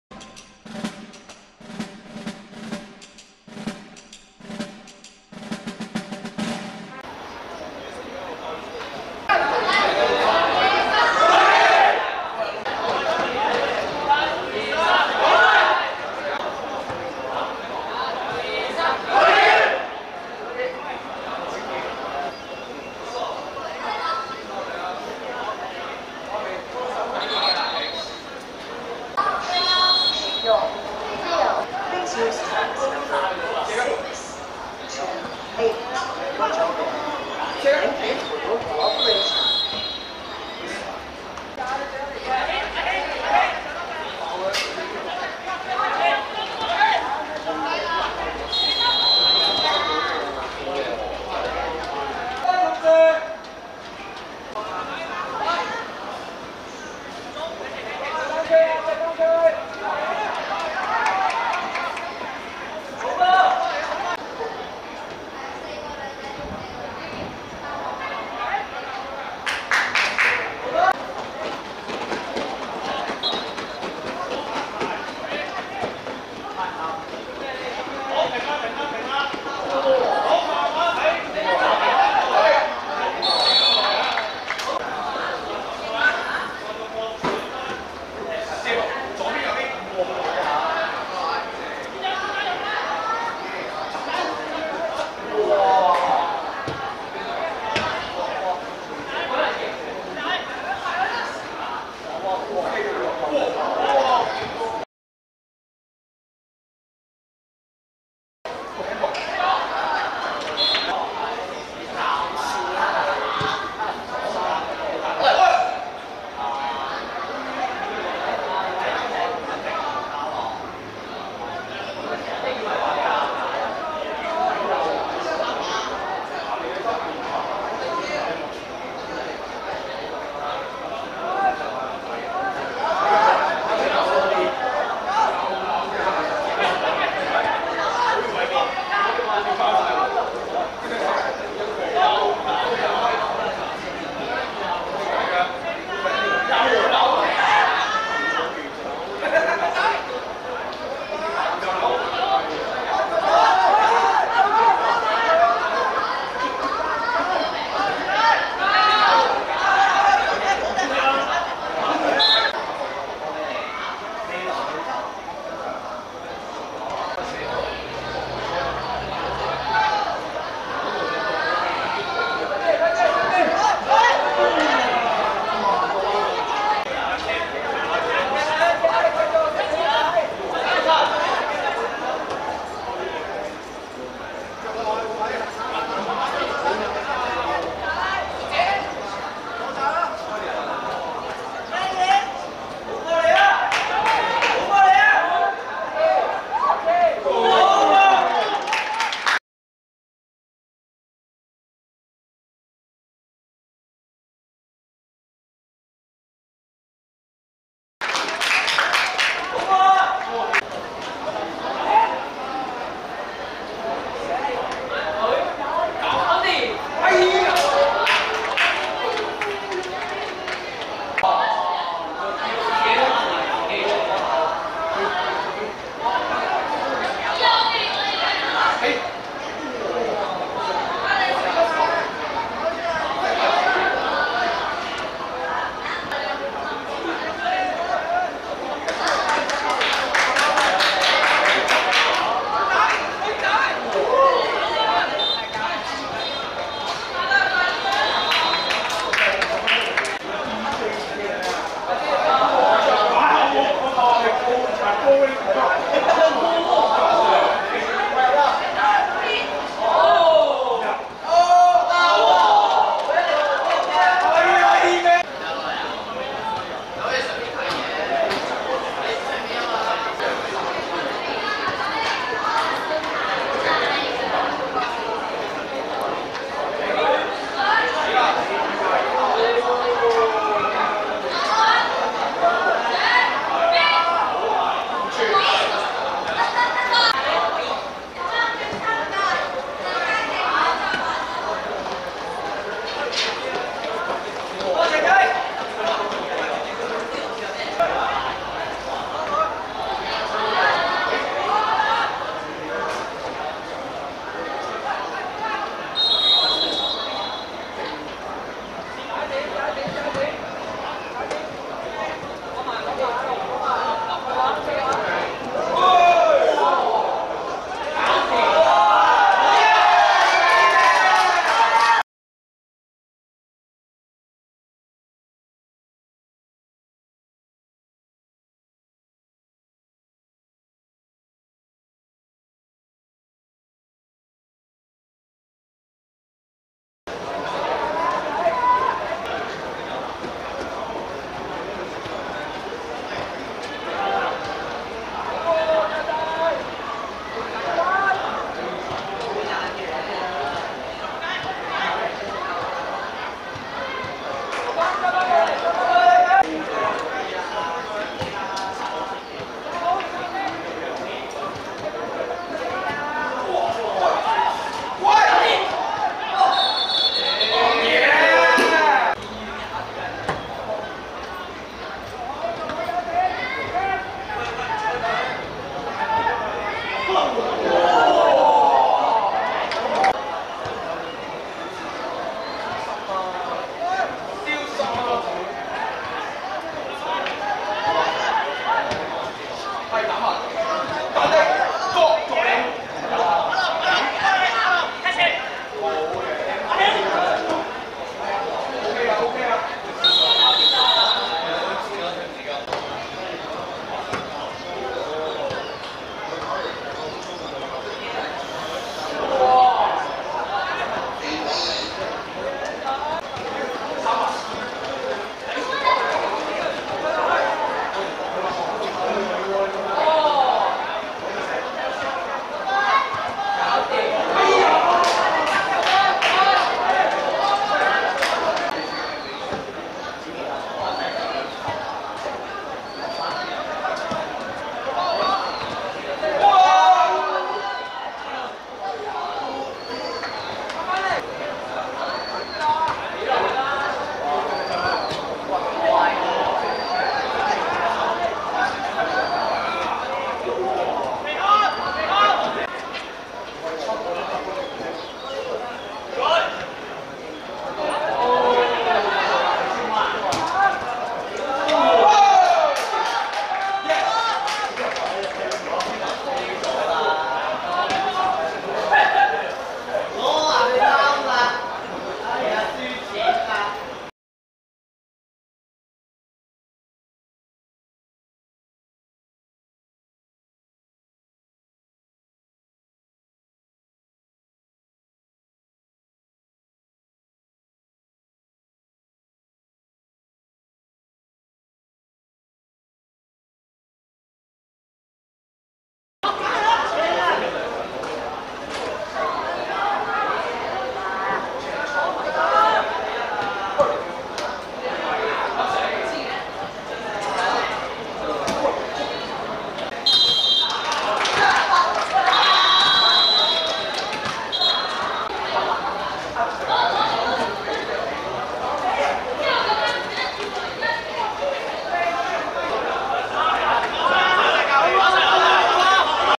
馮梁結vs圓玄二中(2019.9.20.大北區學界足球高級組)精華
2019/9/20 大埔運動塲 16:30 馮梁結(藍) 0 : 2 圓玄二中(白)